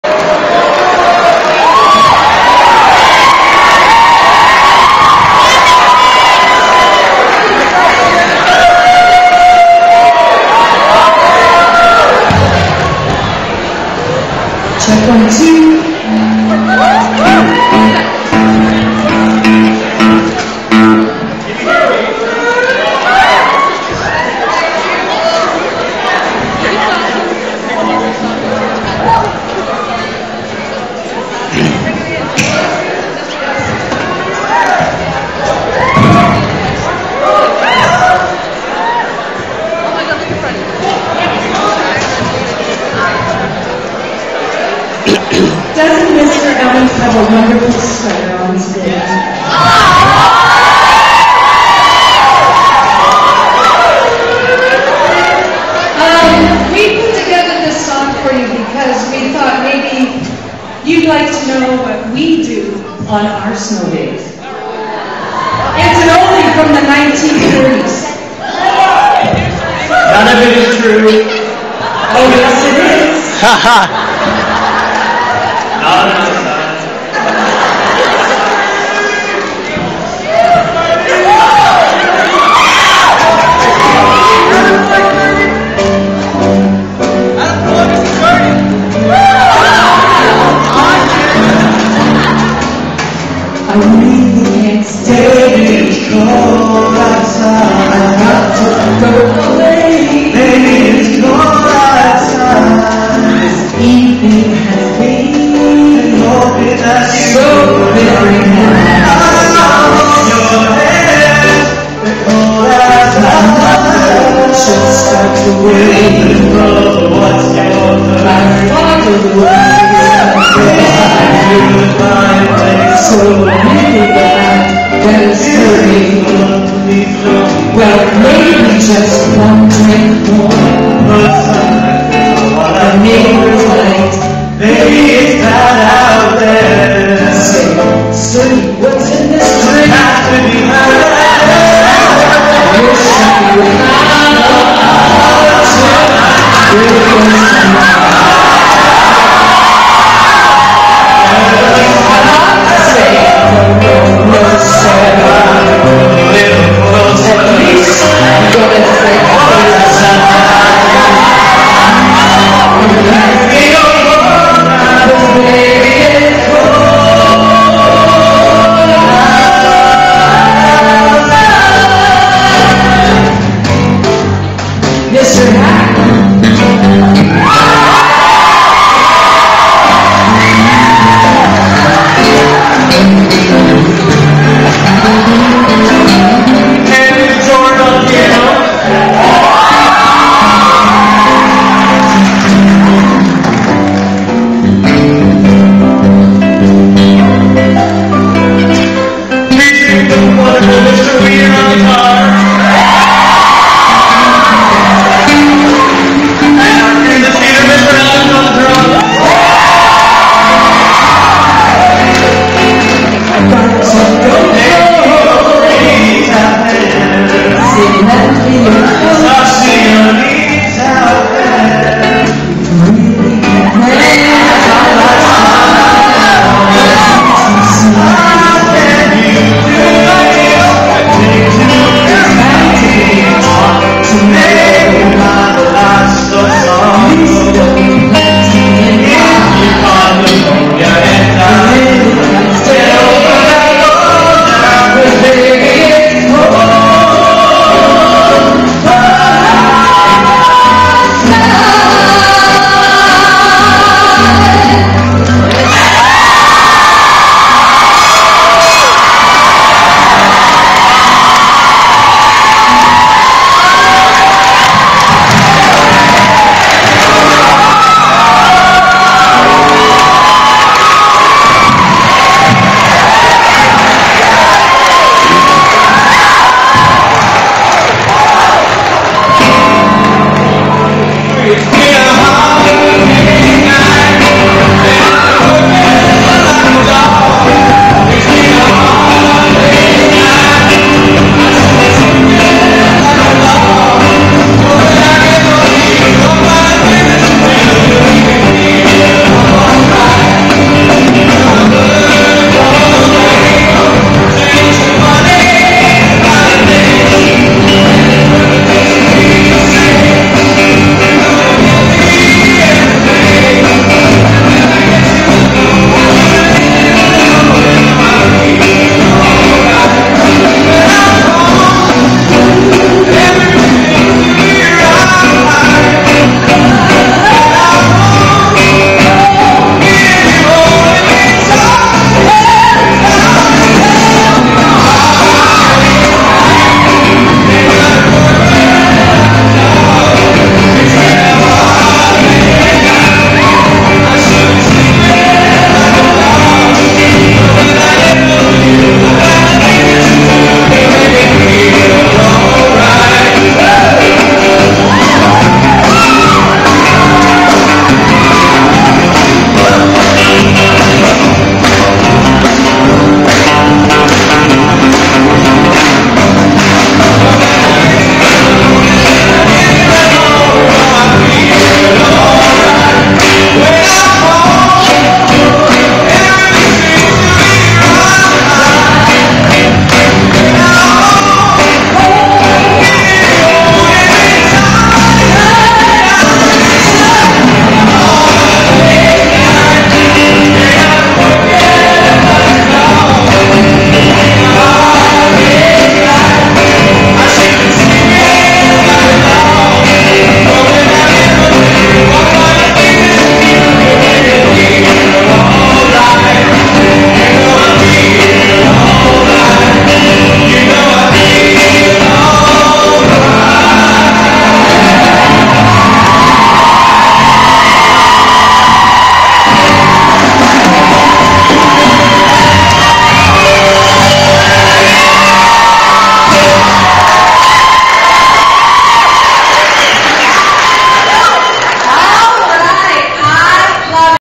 Banting SS Christmas Assembly 2010 Part 5
Music Teachers... and Mr. Weir's Performance